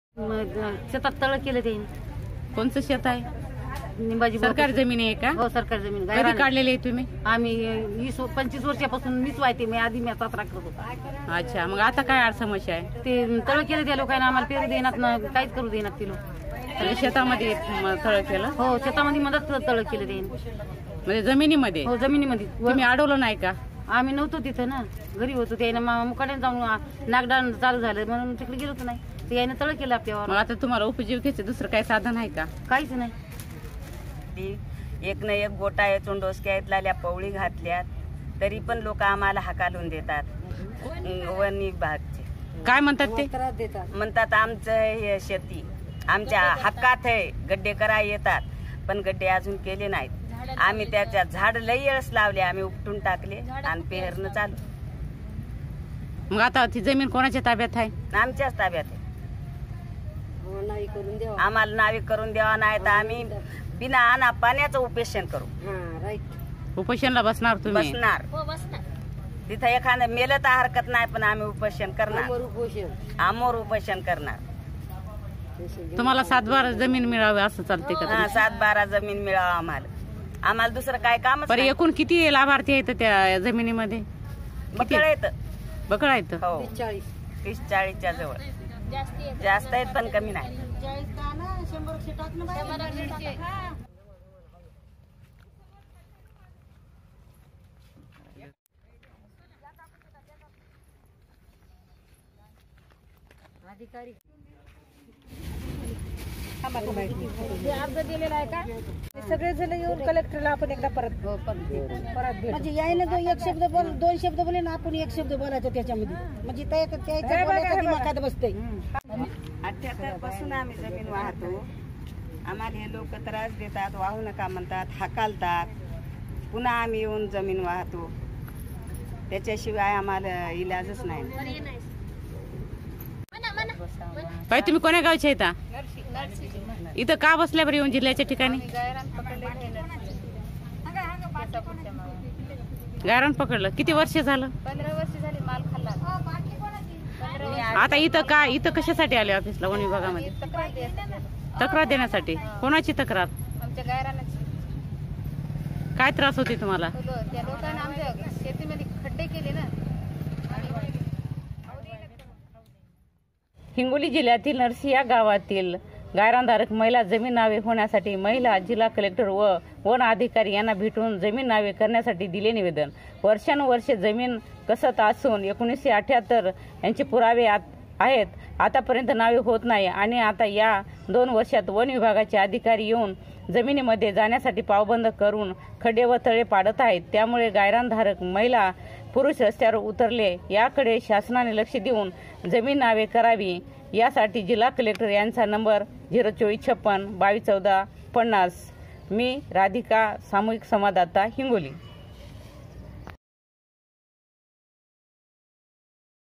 वनविभाग की ओर से जमीन पर अतिक्रमण किया जा रहा है | Forest Department Forcefully Evicting Families
हिगोली जिला के नरसी नामदेव गाव के दलित परिवार के 150 लाभार्थी के सरकारी जमिनीपर पिछले 1978 साल से अतिक्रमण किया है पुरावे भी है लेकीन अभी वनविभाग की और से जमीन पर अतिक्रमण किया जा रहा है|

Community Correspondent Radhika Chicholikar reports from Hingoli, Maharashtra for Video Volunteers.